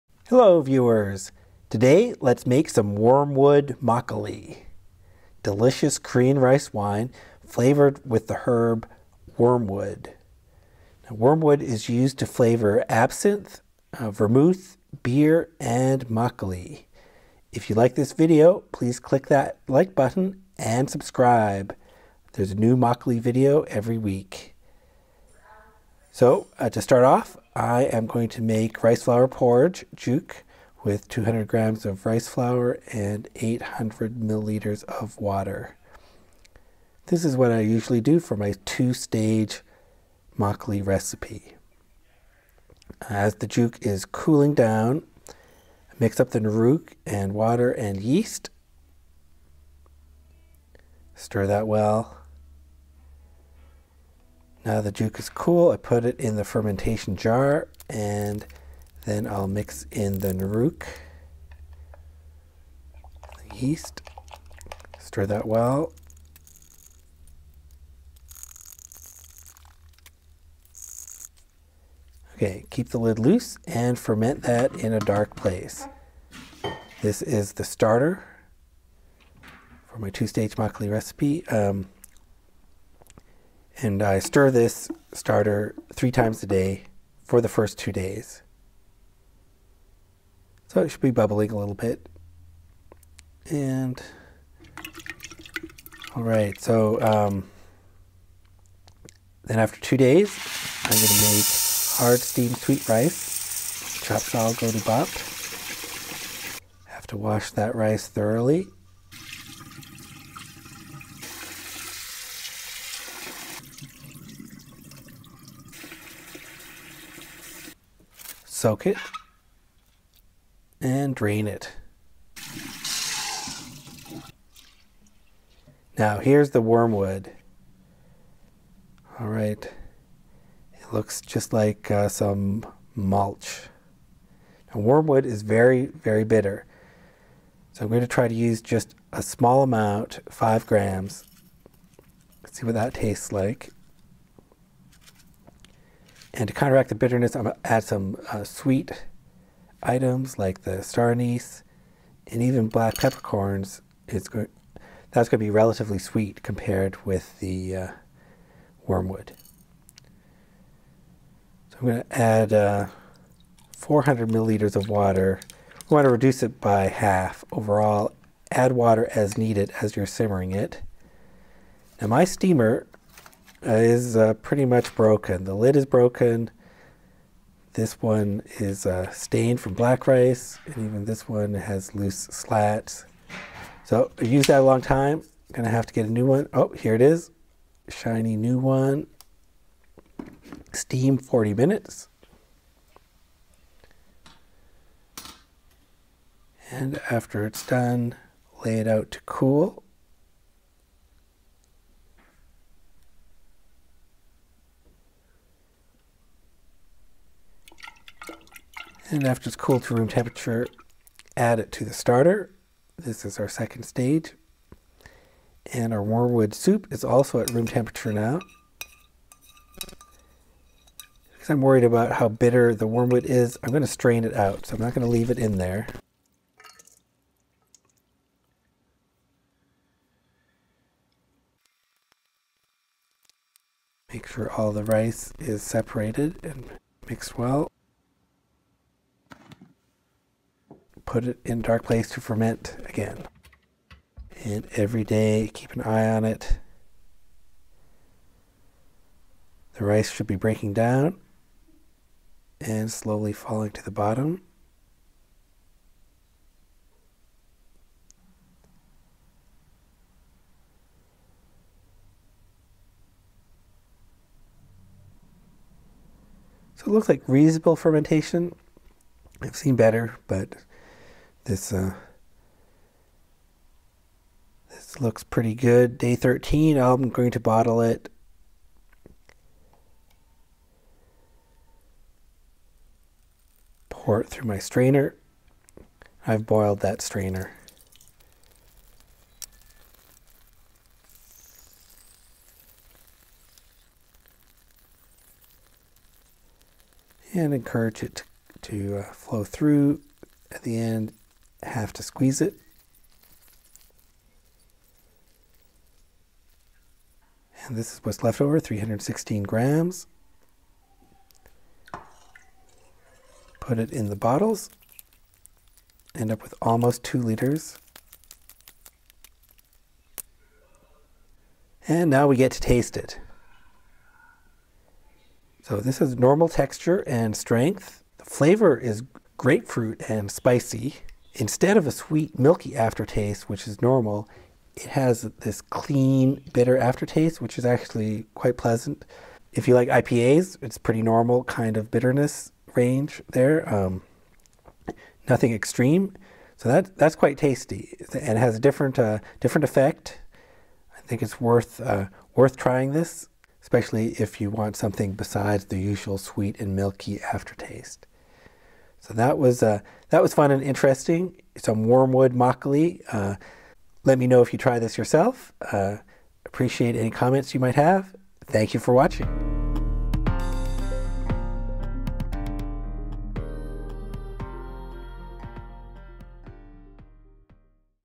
Wormwood makgeolli 쓴쑥 막걸리 homebrew Korean rice wine
Wormwood is very bitter.  Can I brew makgeolli with it?  What does it taste like?